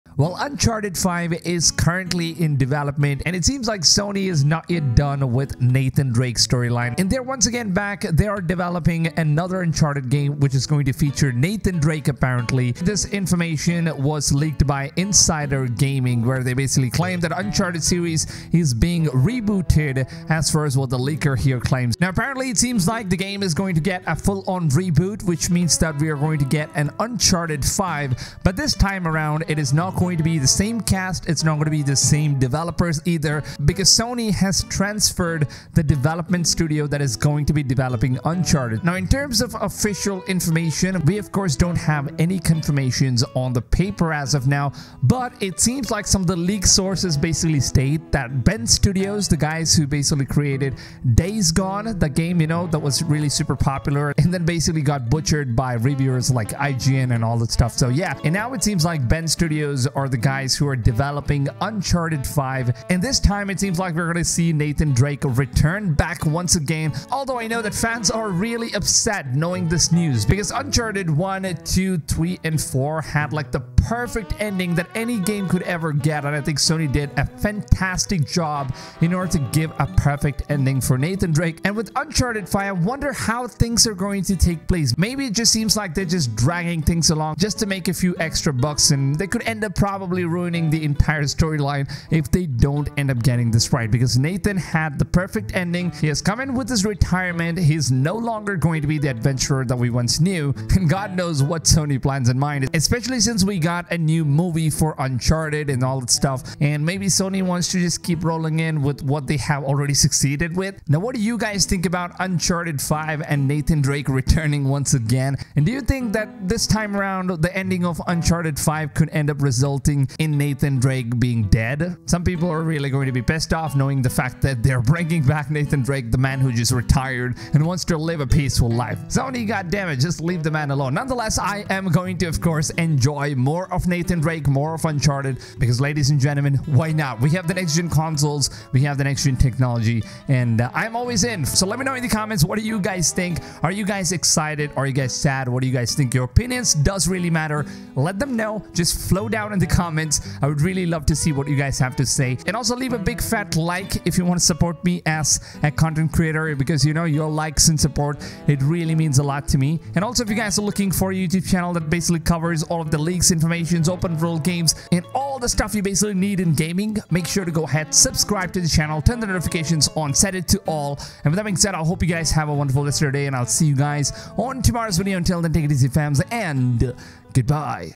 UNCHARTED 5™ | Coming to PS5
Uncharted 5 is coming and it is now confirmed by a leaker and covered by Insider Gaming. It seems like Nathan Drake will make a return this time around to the PS5 with a whole new take on the upcoming Uncharted 5 or also known by the leaker statement as Uncharted Reboot which is currently being developed by Sony but the team leading the game is not naughty dog but a un-revealed development team that is owned by Sony SIE, Now some leakers suspect that the project is lead by Bend Studio who have previously worked on days gone and now could be leading uncharcted 5 with the help of all the dev team at naughty dog that worked on uncharted 1,2,3 and uncharted 4. We will at last see the return of Nathan Drake and his adventures
Today we are going to discuss about how the latest leak features Uncharted 5 or a reboot of uncharted game is out on the horizon and we will discuss about the developers that are making the game, along side sony's plans on when this could turn into an official information anytime soon.